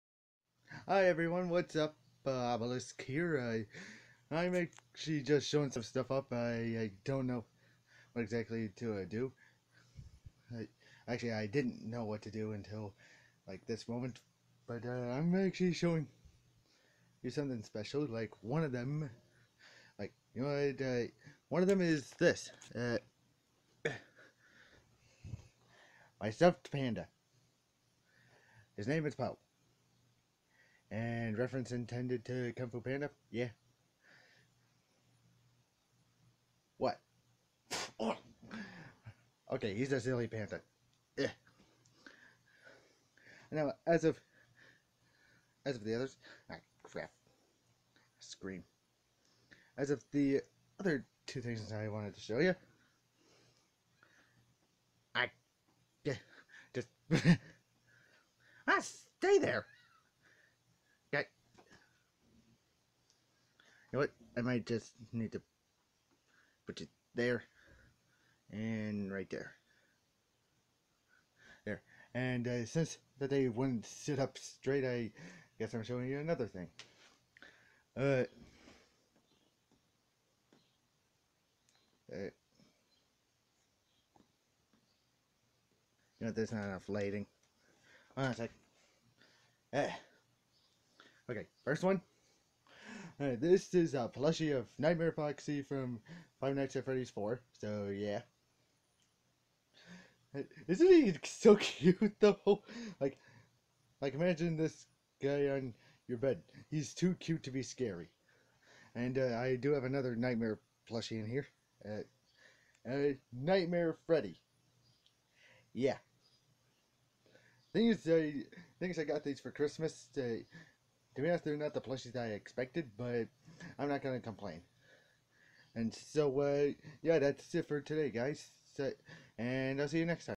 My stuffed buddies
I didn't know what to do so I decided to show off my stuffed friends and I hope u like them.